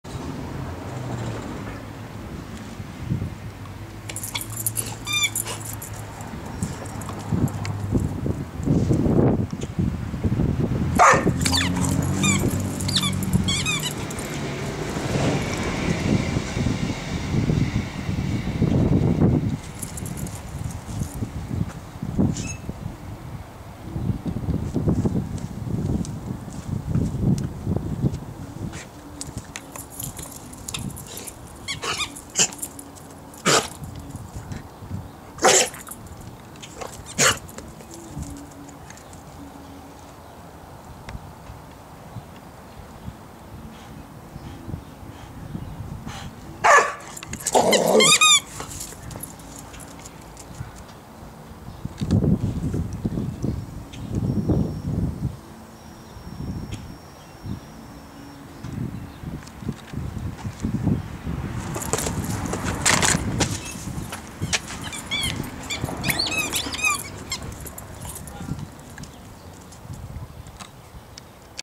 Zero, the choking Brittany Spaniel (in HD)
Here's a short video of my dog, Zero. At one point he chokes and at the end I trip over the tri pod. But it's in High Definition!